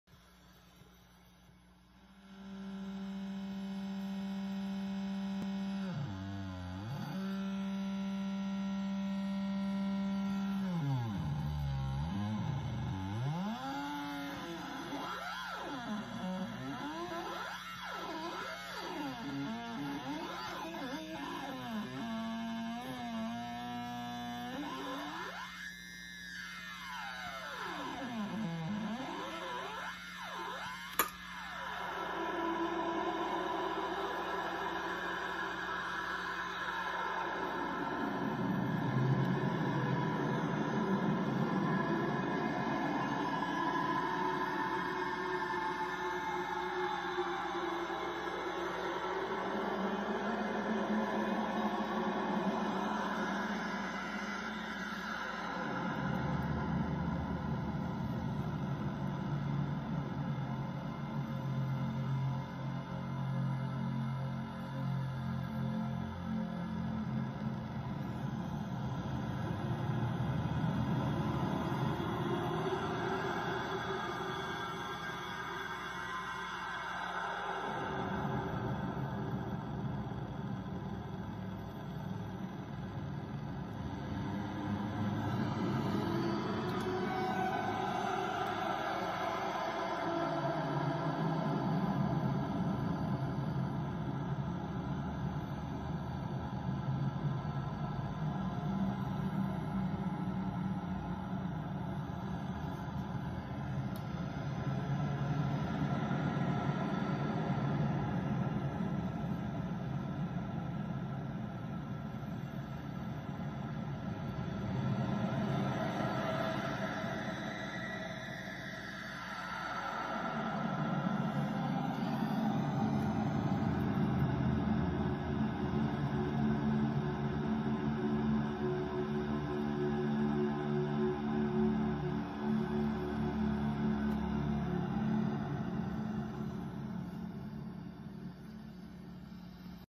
Drone jar
Playing with the Michael Rucci Drone Jar.  It’s got three light sensors that determine the amount of signal being fed into it. Changes tones and sounds based on types of light as how much is let in.  Processed through Walrus Fathom and EQD Dispatch Master.